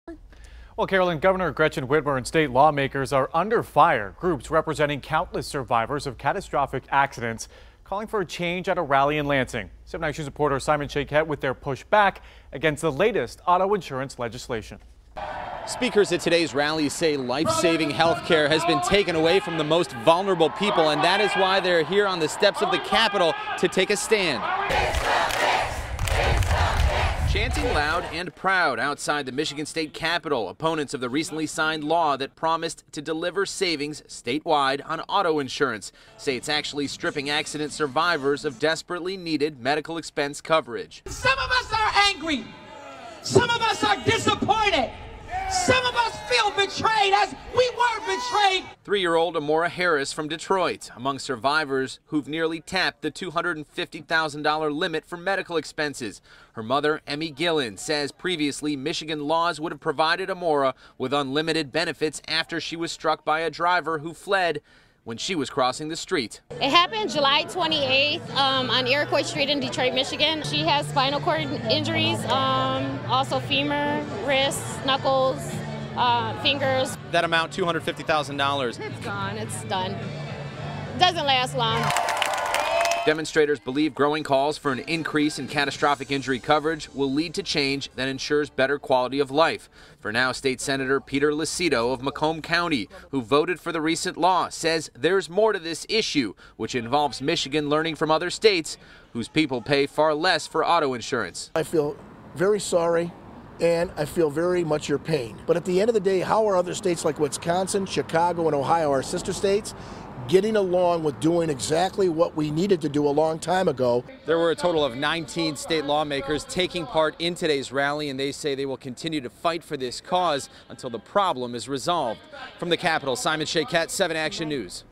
Rally for change in auto insurance coverage at state capital in Lansing
Thousands of people gathered in Lansing on Wednesday morning for a rally about no-fault auto insurance.